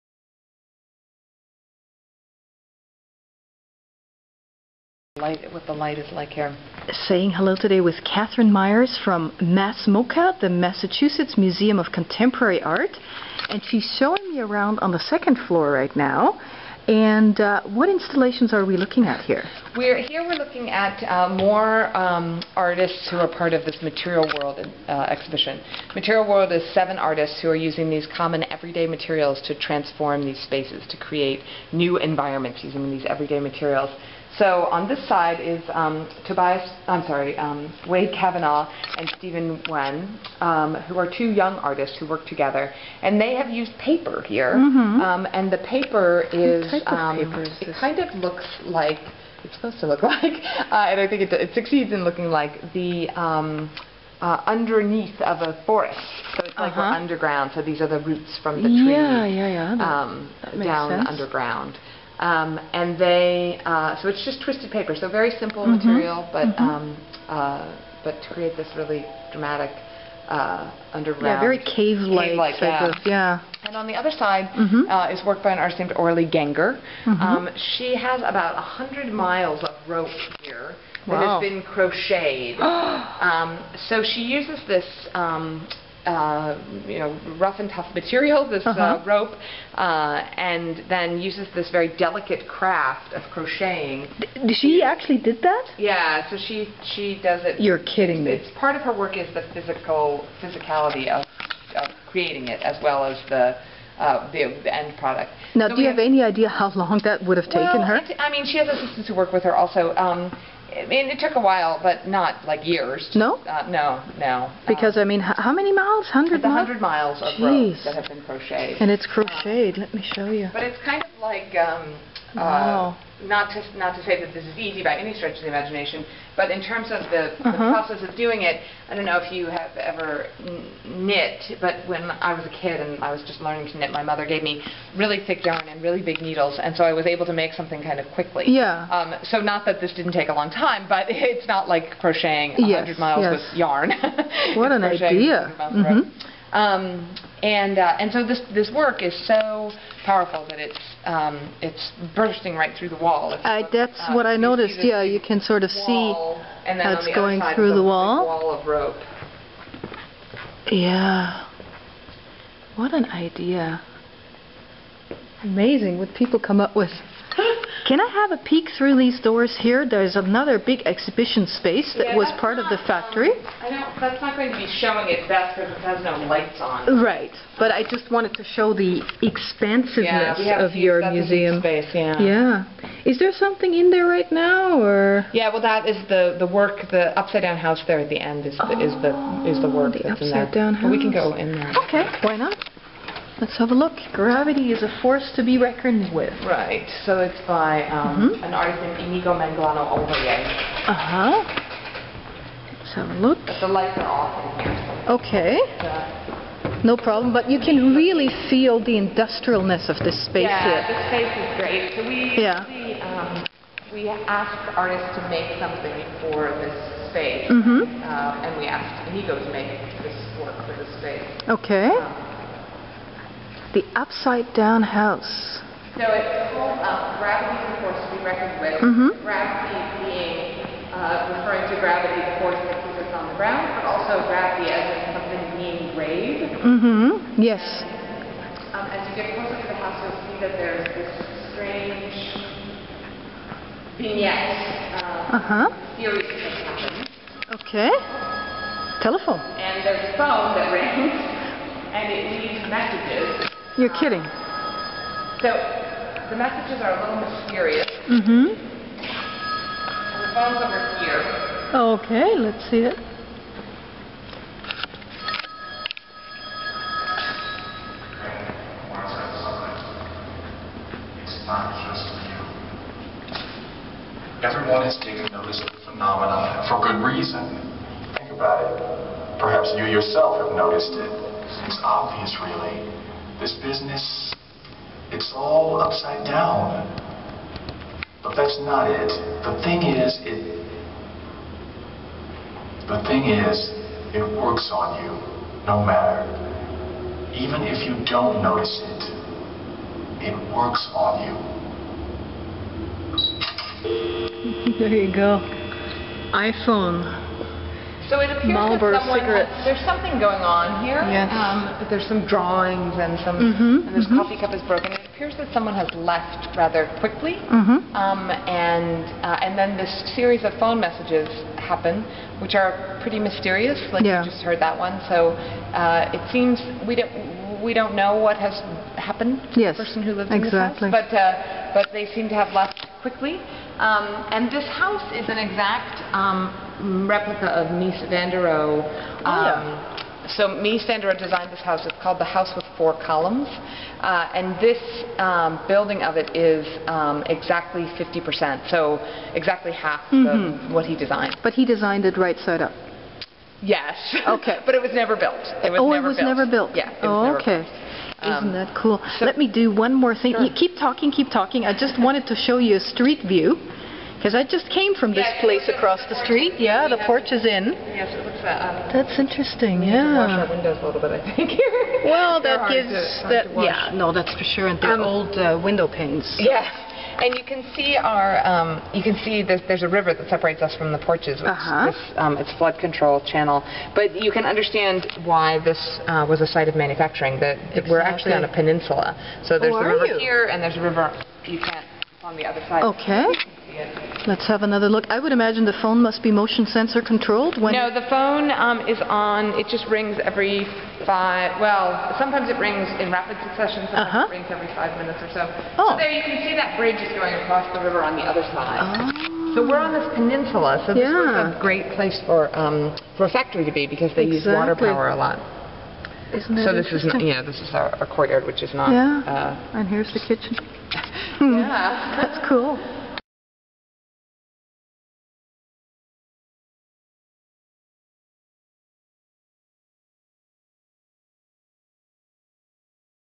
Massachusetts Travel: Discoveries in the Berkshires -- North Adams and Williamstown
On September 28, 2010 I headed off to the town of North Adams to explore another award-winning historic structure: the Porches Inn, a collection of Victorian row houses, former mill workers' homes, that was lovingly rescued and restored by successful local hospitality entrepreneur Nancy Fitzpatrick. Nancy showed me through the entire property which features 47 uniquely appointed rooms that charm the visitor with their "industrial granny chic ambience". Sabine, the housecat, welcomed me as well, and as a true architecture lover, I was duly impressed by Nancy's commitment to historical preservation.

Across the street from the Porches is MassMOCA, the Massachusetts Museum of Contemporary Art, the largest center for contemporary visual art and performing arts in the United States. Housed in huge industrial complex that was formerly the Sprague Electric Company, contemporary art installations have found a perfect home here. Starting with 5 upside-down trees in the courtyard, this museum offers a provocative art experience. Under the title of "Material World: Sculpture to Environment", I found large-scale art installations that were composed of fishing lines, twisted paper and crocheted rope that dominate the expansive spaces of this former factory. Under the title "Gravity is a Force to be Reckoned With", the "upside-down house" leaves the visitors with more questions than answers. And Sol LeWitt's collection of 105 colourful oversized geometric wall drawings provides a visual feast of colours and shapes.

Then I embarked on a walk through North Adams, a former industrial town whose 19th century economy flourished with textile mills while the mid 20th century was dominated by electrical components manufacturers. From the mid 1980s onwards, most of the major manufacturing companies had shut down and sent the town into an economic tail spin. Over the last few years, North Adams has successfully started to reinvent itself as a center of arts and tourism whose anchor is the Massachusetts Museum of Contemporary Art. A pretty main street and downtown area is augmented by several Victorian-era mill buildings that have been converted into artists' studios, galleries and loft spaces. Art lovers are starting to flock to North Adams for its cultural attractions.

Just west of North Adams I visited the picturesque college town of Williamstown, home to Williams College, one of the northeast's best private liberal arts colleges. The college's campus features a large number of attractive buildings, and arts enthusiasts come here for the Clark Art Institute and the award-winning Williamstown Theatre Festival.